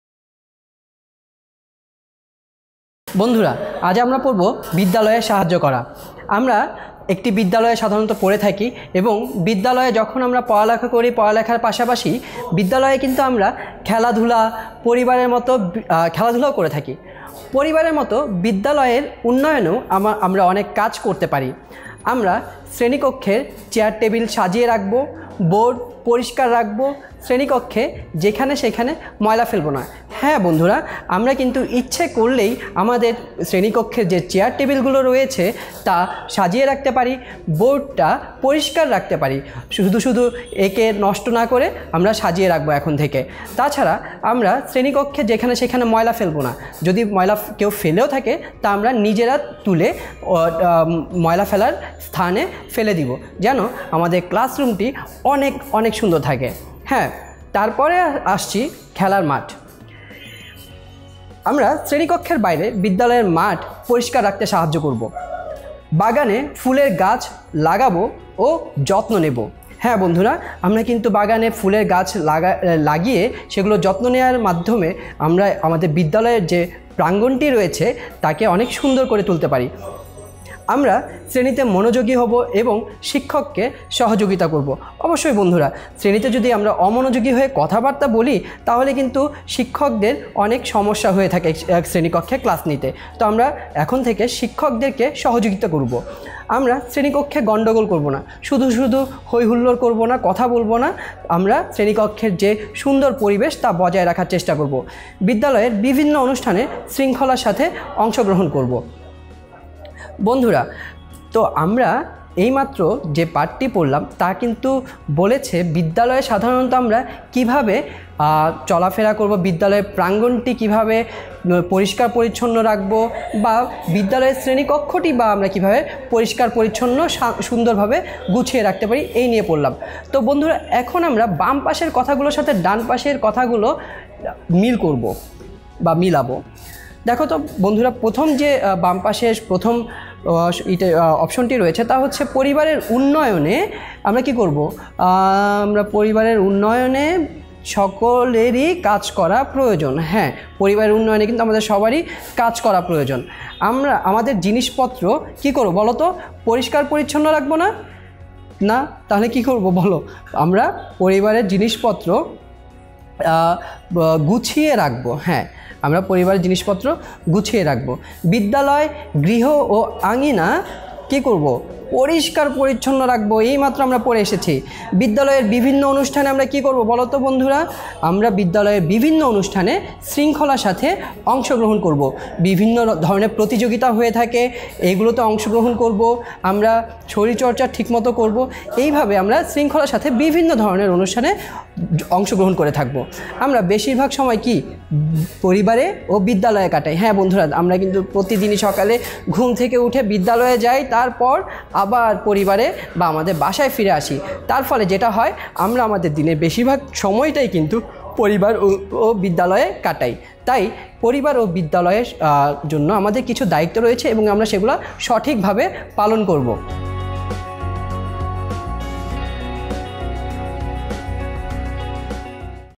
০৬.০৩. অধ্যায় ৬ : সামাজিক পরিবেশের উন্নয়ন - পাঠ ৩ বিদ্যালয়ে সাহায্য করা - [Class 3]
টেন মিনিট স্কুলের প্রোডাক্ট ও কোর্স সম্পর্কে বিস্তারিত জানতে কল করো:📞16910

Series: ৩য় শ্রেণি বাংলাদেশ ও বিশ্ব পরিচয় (Class 3 Bangladesh & Global Studies)
Instructor: Abdullah Al Mamun